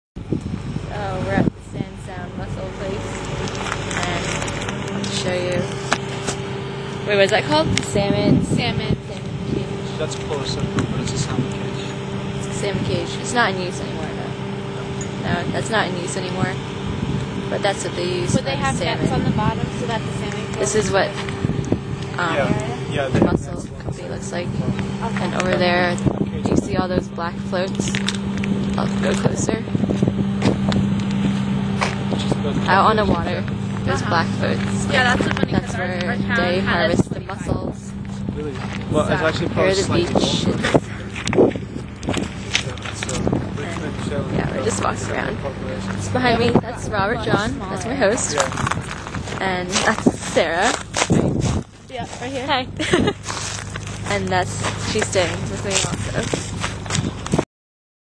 Shetland Salmon Cage
Visiting the beach in Shetland with RJ